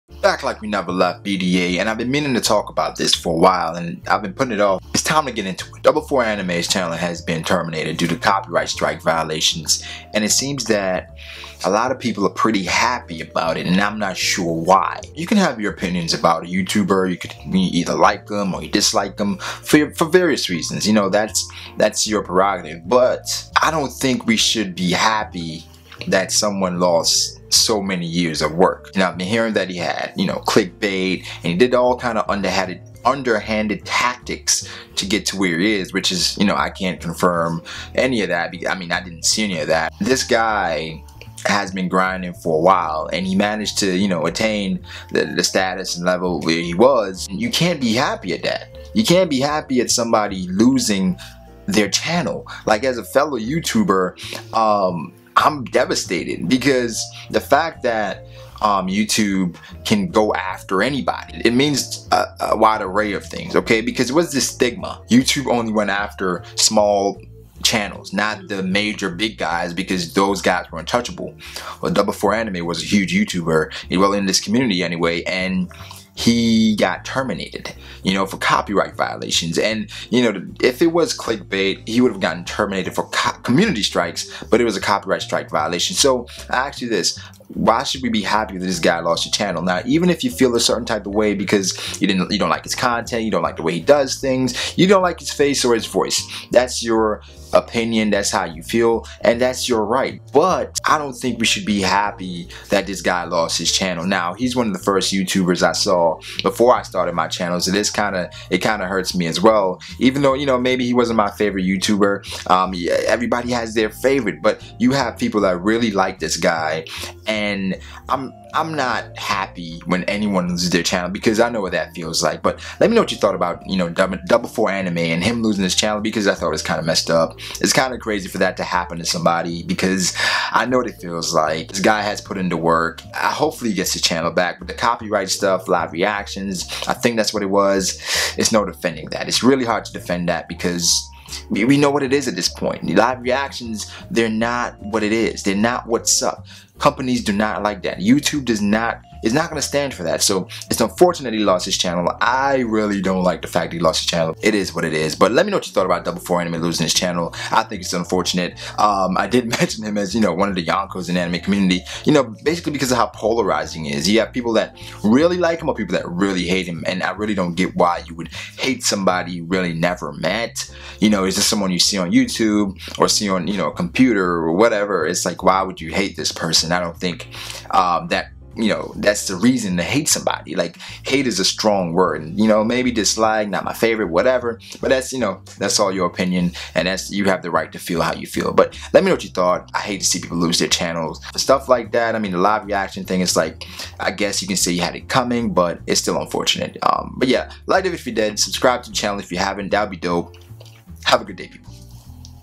Double4Anime's Channel Is Terminated & People Are Ecstatic! Why?
Just curious as to why people are so happy about a channel termination. More after the click.